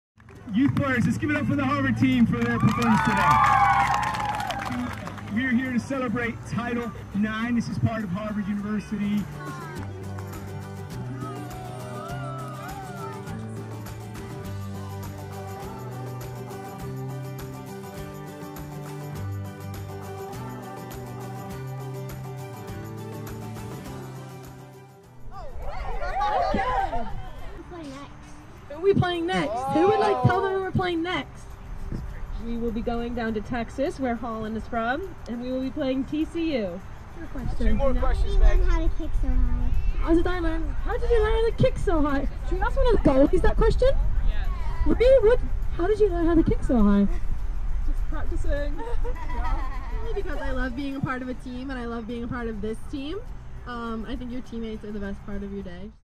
Women's Soccer Celebrates Title IX with Postgame Youth Clinic
Harvard women's soccer continued Harvard Athletics' 50th Anniversary celebration of Title IX by hosting a youth clinic after its win over the Massachusetts Minutewomen on Sunday, September 11, 2022.

The team taught the youth players the fundamentals of soccer before answering questions and taking pictures with the group.